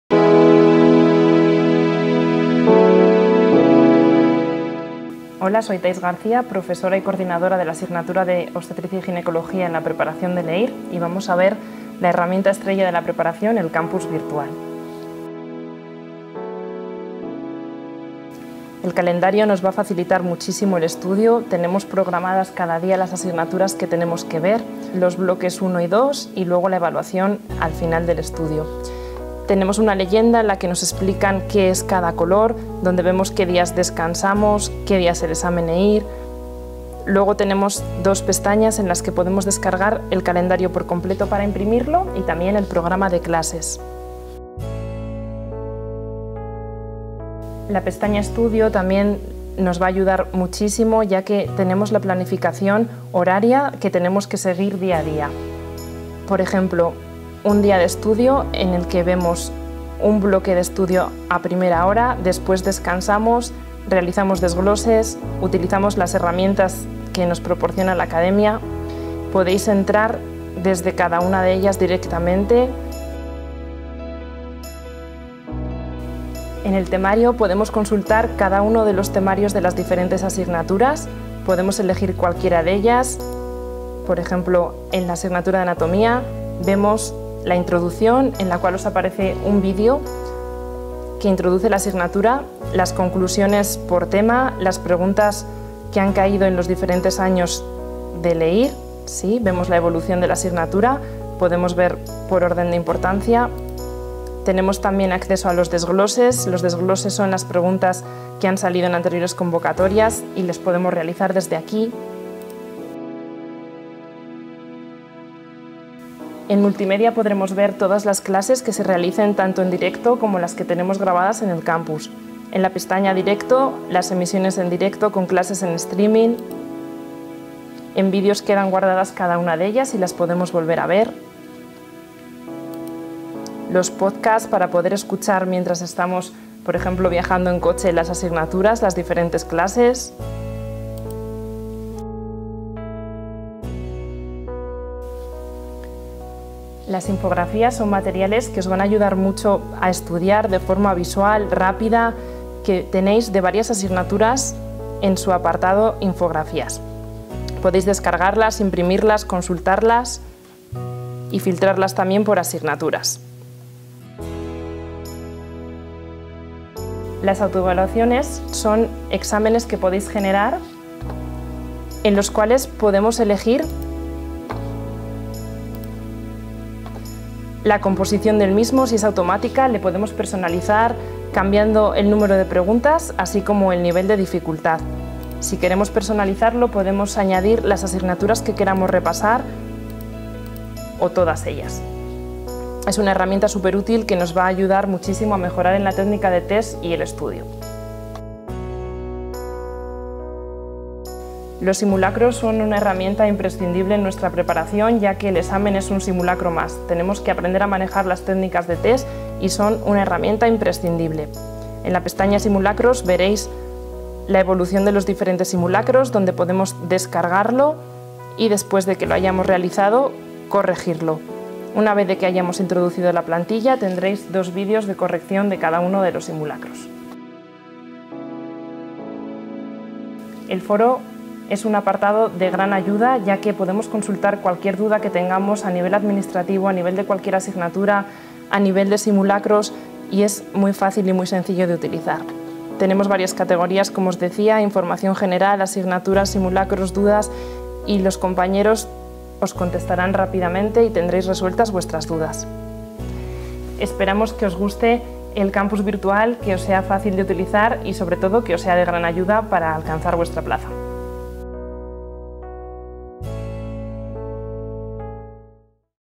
Herramientas Campus EIR Grupo CTO
La profesora de CTO Enfermería, Thais García, explica las diferentes herramientas del campus virtual del curso EIR de la academia. Entre ellas destacan el calendario de estudio, el temario, los recursos multimedia y los simulacros.

Aquí tienes toda la información del curso EIR de CTO Enfermería